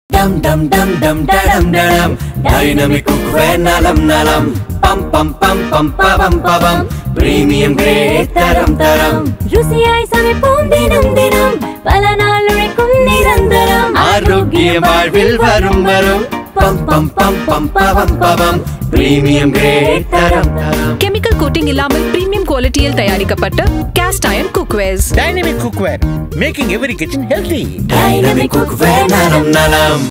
Dynamic Cast Iron Cookwares | Jingle TVC | Smart Work Productions | Chef Damu | Smruthi Venkat | 4K
Client: Dynamic Cookwares
Production House: Smart Work Productions
Direction: Manoj
Creative Head: Dhanasekar
Cast: Chef Damu, Smruthi Venkat, Baby Krishitha
Direction Team: Shankar, Nithish Kumar
DOP: Prasanna S Kumar
DOP Team: Sundar, Mani, Pravin, Mani, Abhishek
Focus Filler: Venkat Arikatla
Choreographer: Sara Master & Team
Costume & Styling: Sathya NJ
Makeup: Suresh & Team
Art Director: Appu
Food Stylist: RK
Production Manager: Hari Hara Sudhan, Sasi
Editing & Online: Dhanasekar
Music & Mix: Voicceestore
Singers: Rita, Gopal, Smrti, Sai Shradhaa
Colorist: Sheik (Color Pixels)
CG: Satheesh
Photography: Jose, Ragaven
Making Video: Dilip Balasubramaniam
Smart Work Team: Mary Sugandhiya, Nandhini, Nithya Sekar, Hari Sudhakar, Siva Kumar, Saravanan, Saravana Kumar, Pragasamoorthy